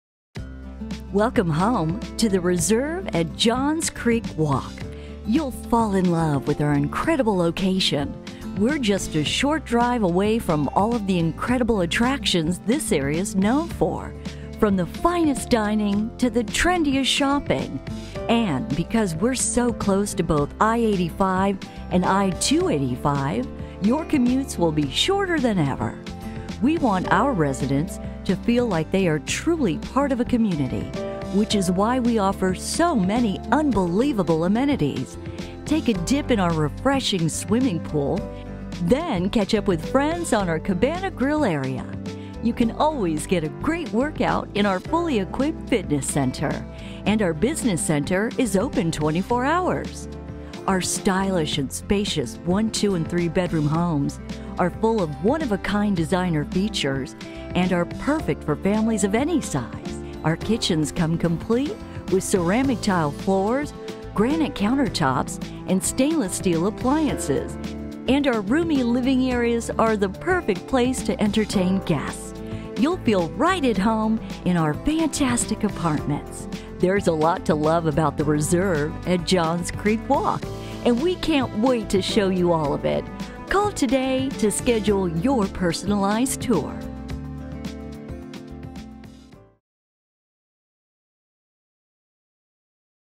The Reserve at Johns Creek Walk Apartments in Johns Creek, GA - ForRent.com
Luxurious residences with remarkable interiors provide comfort, elegance, and un-matched convenience.  Natural surroundings create a tranquil enclave with native trees and flowers, community parks, walking and biking trails, and a spectacular swimming pool with a cabana grill. -- The Reserve at Johns Creek Walk is complete experience.  It was created for a spirited life, your way - and it's different from anything you've ever known.  In fact, it's difference between living, and living well.